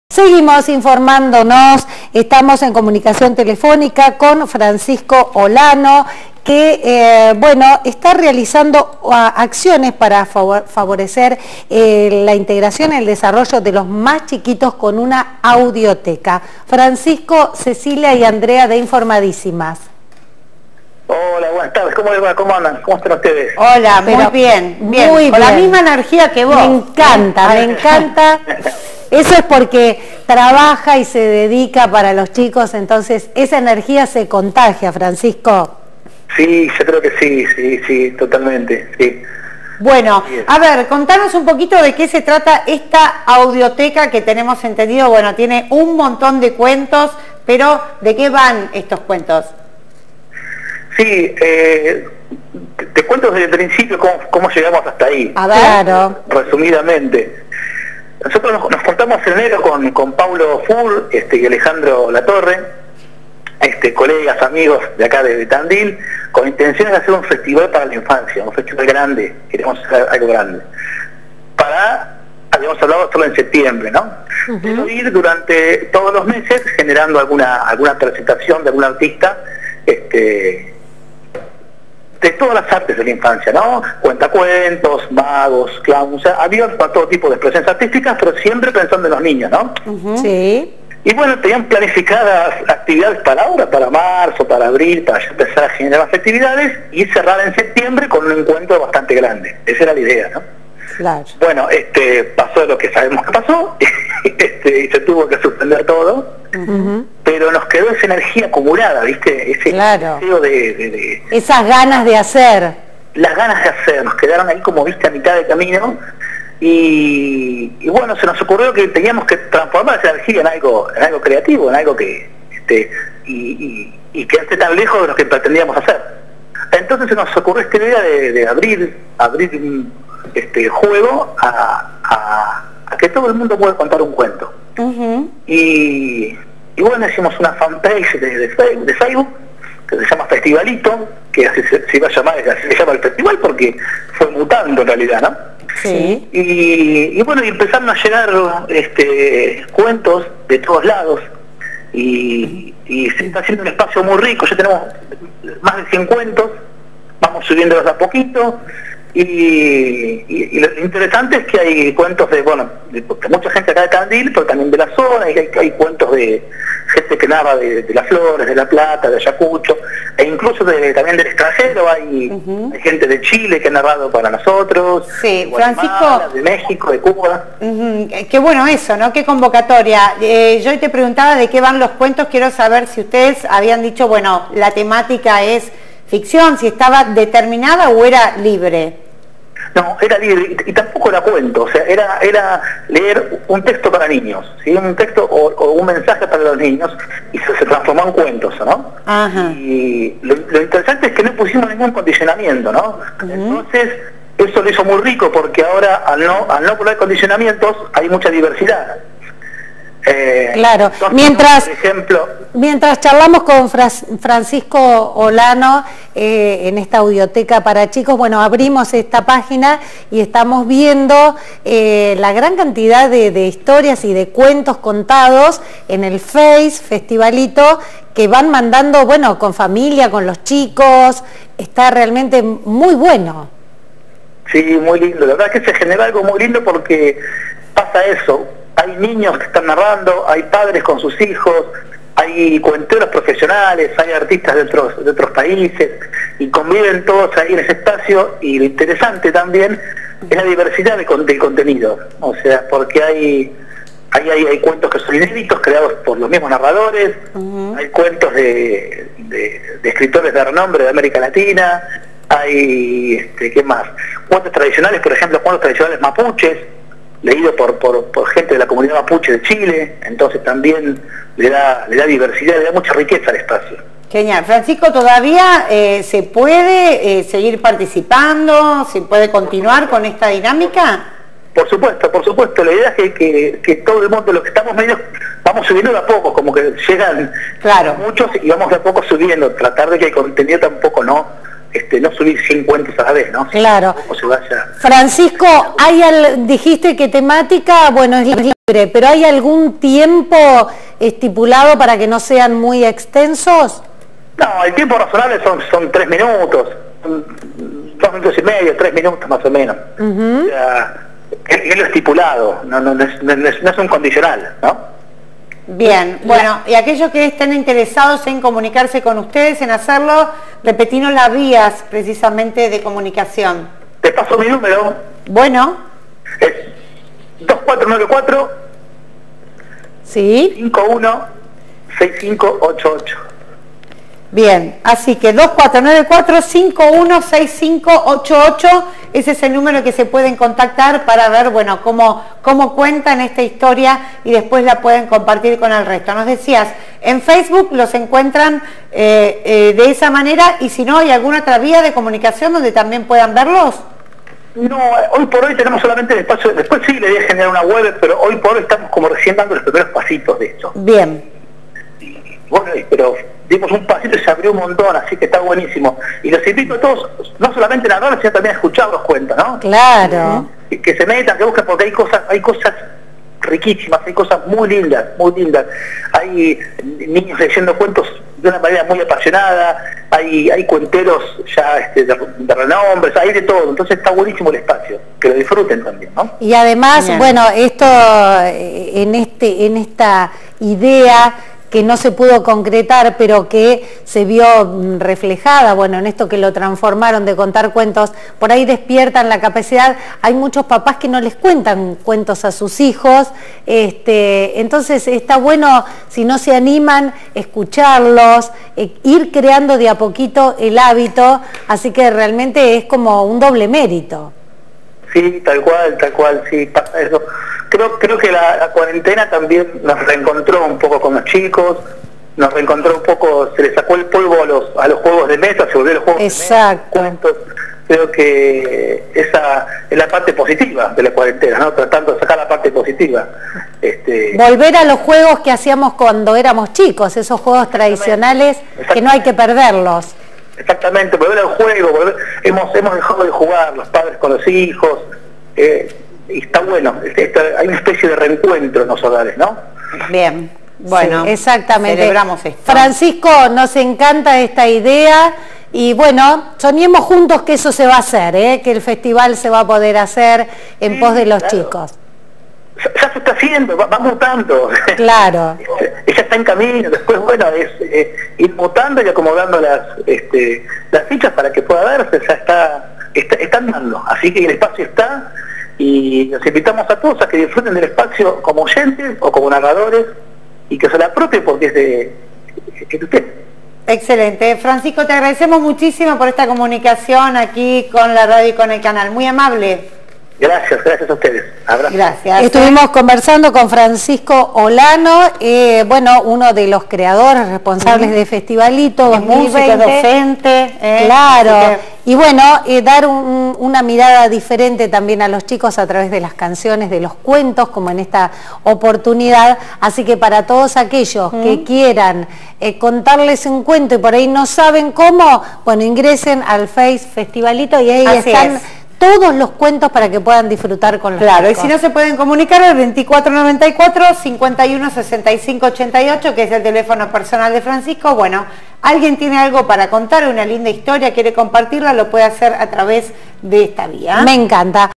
Se realizan acciones para favorecer la integración y el desarrollo de los mas pequeños
Francisco Olano - Audioteca para niños dialogó con Informadisimas por Eco TV y la 104.1 Tandil FM.